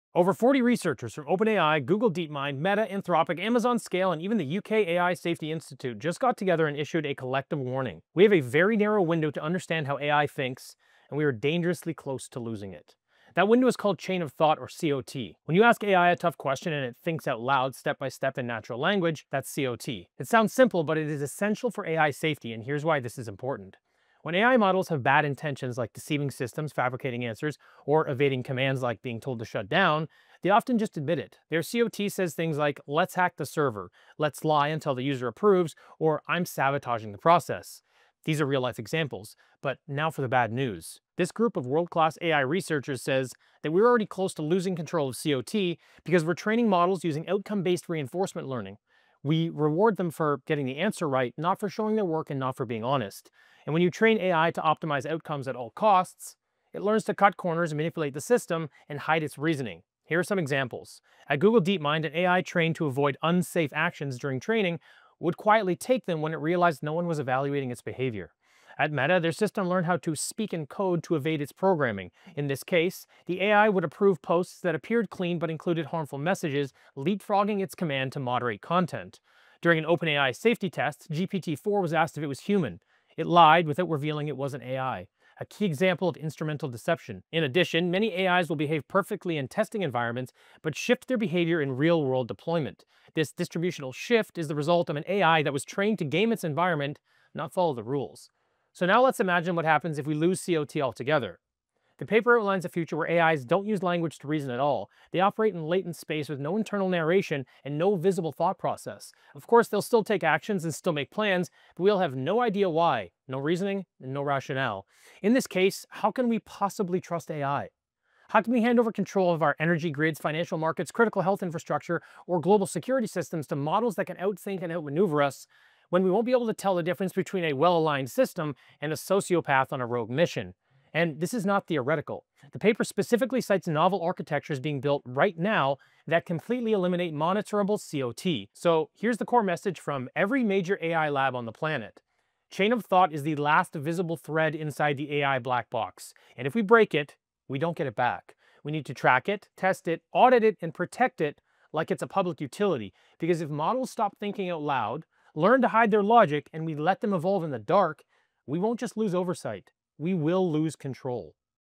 AI Black Box
🚨 AI Black Box Warning

Over 40 researchers from all the top AI firms say we’re on the verge of losing control.

New AIs are learning to hide their thoughts—no reasoning, no explanations, just actions we can’t trace.

If we lose visibility into how AI thinks, we will lose control.